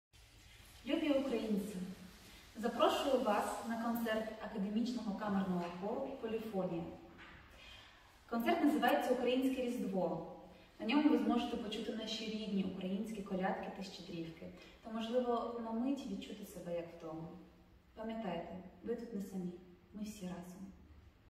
Kate Tomchuk kvietimas
Sausio 10 d. 18 val. Kelmės kultūros centre Ukrainietiškos Kalėdos.